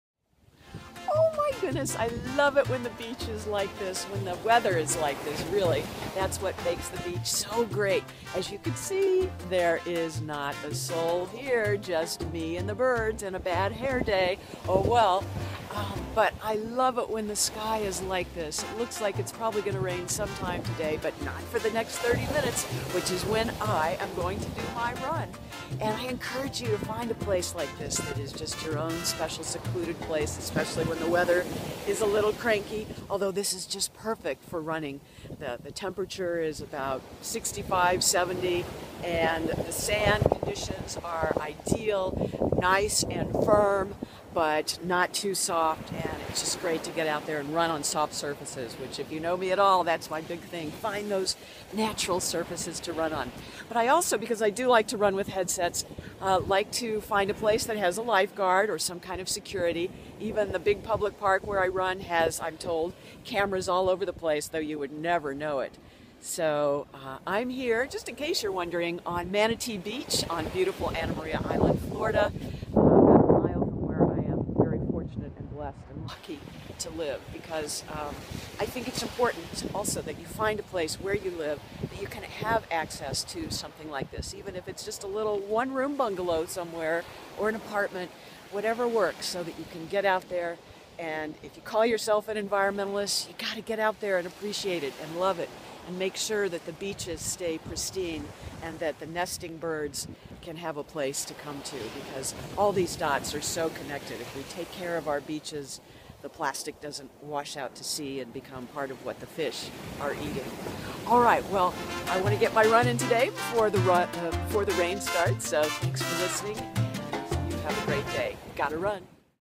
Find Your Place in the Sun or Clouds
Making fitness a priority can be a challenge if it isn't in your backyard, or close to it. "Eat Vegan on $4 a Day" author Ellen Jaffe Jones gives tips on turning a cloudy day into a fit day!